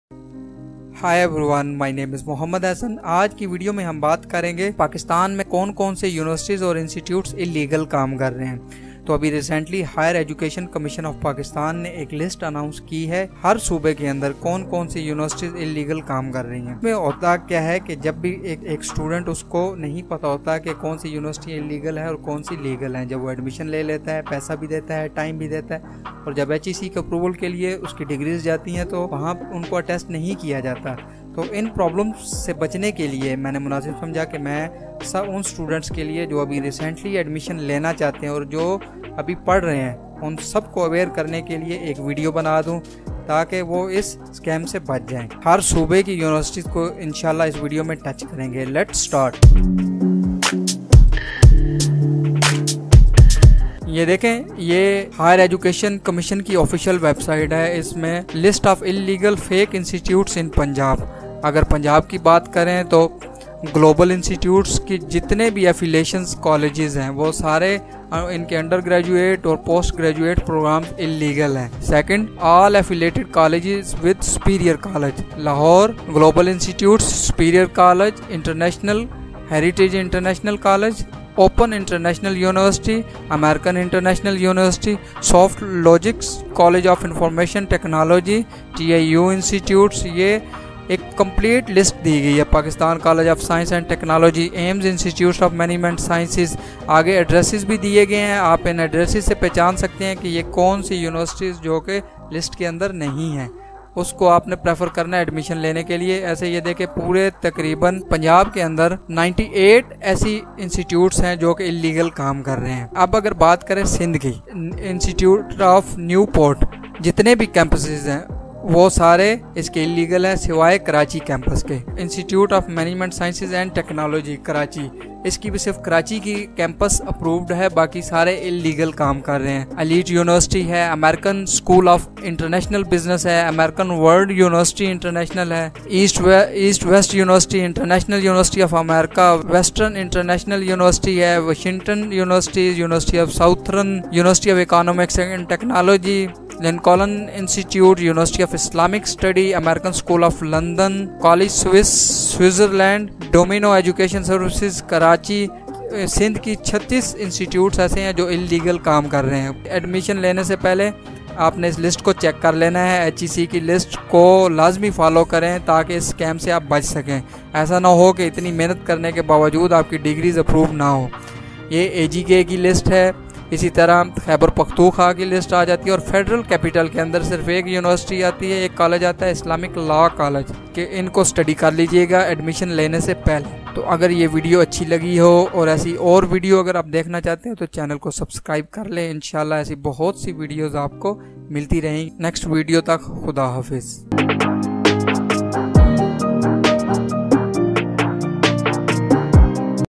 HEC Illegal / Fake Universities & Campuses in Pakistan [Punjab - Sindh- KPK - AJK]
Any other institution which is not included here or  in the list of recognized universities/Institutions/Campus (s) available on the HEC website may also be termed as non-recognized.​​​​​​

Recently, the Higher Education Commission issued a list of recognized and fake or illegal universities operating in the country. HEC warned students that the latter institutions\’ degrees would not be recognized.

List of Illegal/Fake Institutions in​ Punjab
​List of Ilegal/Fake Institutions in Federal Capital
List of Illegal/Fake​ Institutions in AJK​
List of Illegal/Fake Institutions in Sindh